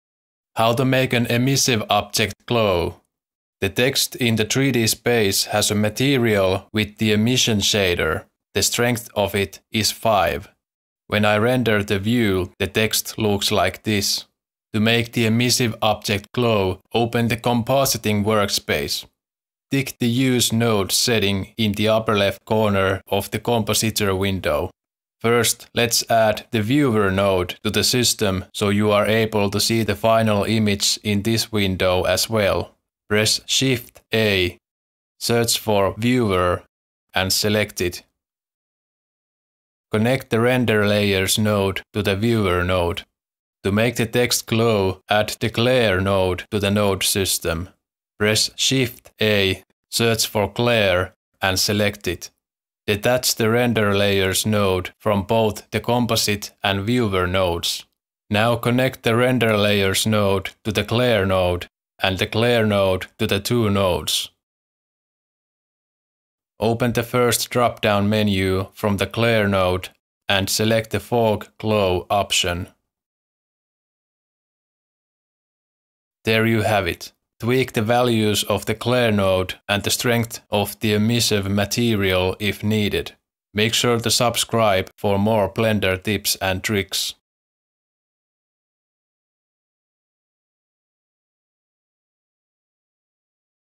How to Make an Object Glow in Blender
This tutorial teaches you how to use the compositing workspace.

Using the glare node in the compositing workspace allows you to make the emissive objects in the 3D space glow. This increases the realism of the render quite a lot.